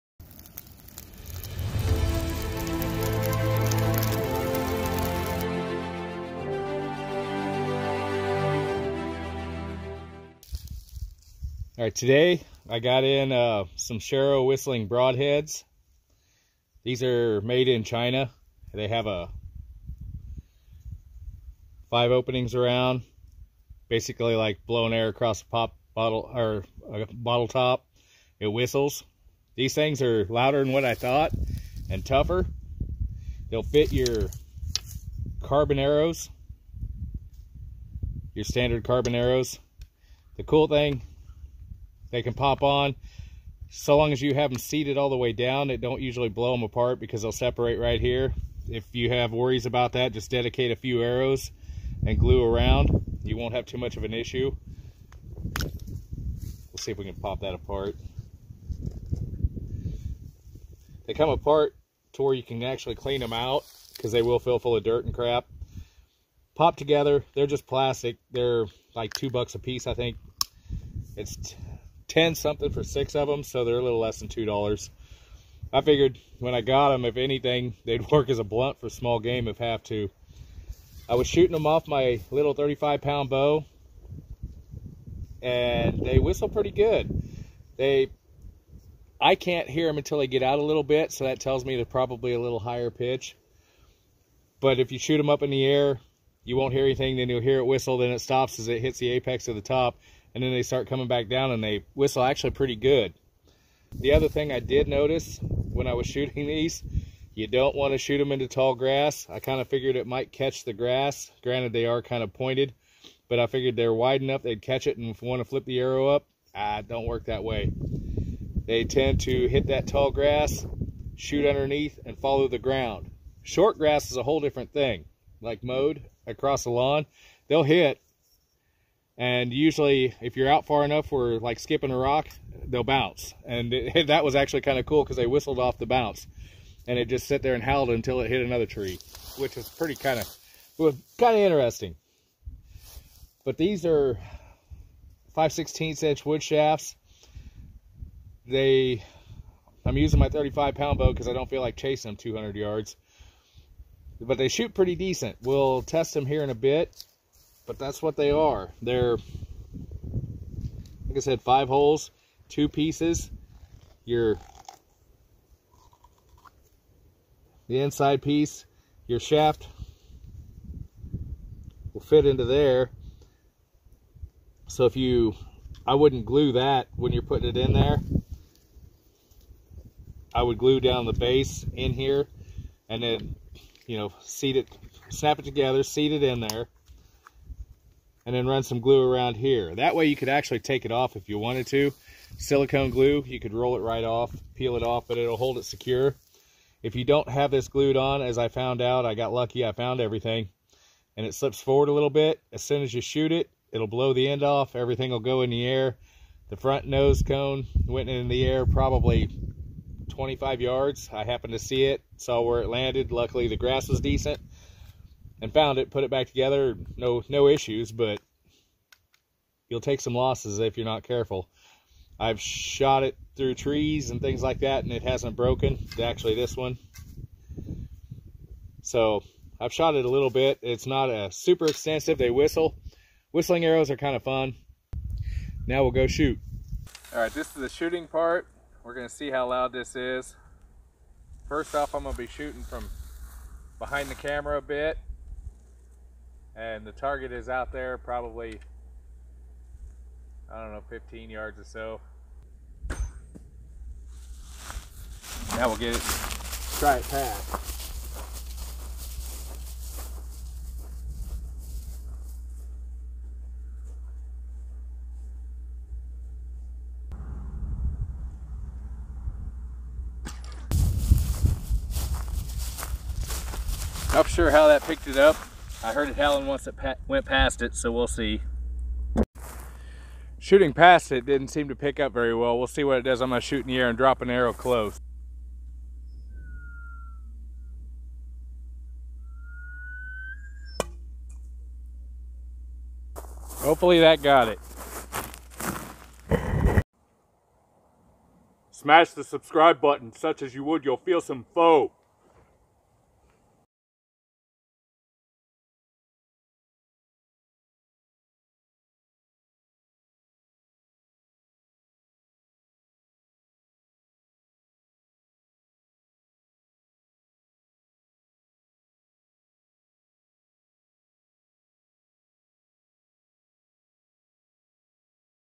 Odin's Wolf Survival Reviews Sharrow Whistling Arrowheads
These are a plastic low cost arrowhead that works quite we).